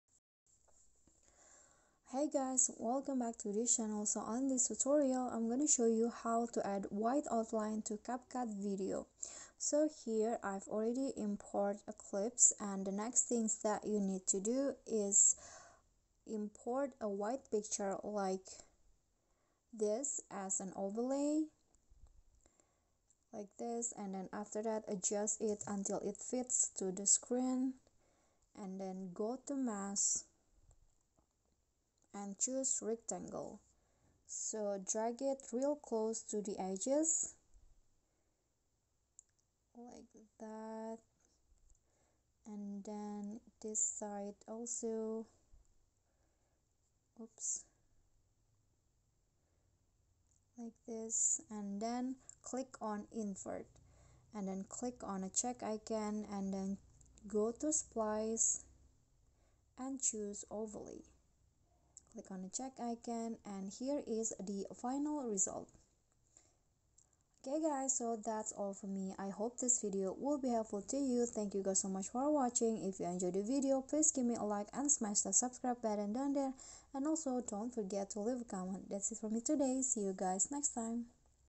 How to Add White Outline to CapCut Video
Here is how to do white outlining in CapCut